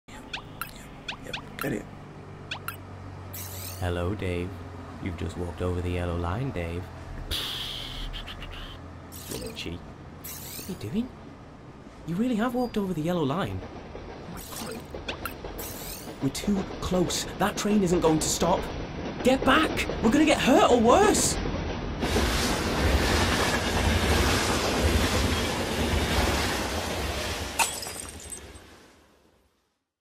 Uclan Animation Channel Arriva North Safety video - Yellow Line
Year animation students worked with Arriva North's David Wilkinson to create this series of safety video's for school.  Ashley Collier designed the whole look and also directed this one. Uclan acting students did the voices.  Uclan screenwriting students wrote the scripts.  Uclan sound design students mixed the sound.  A Uclan production!!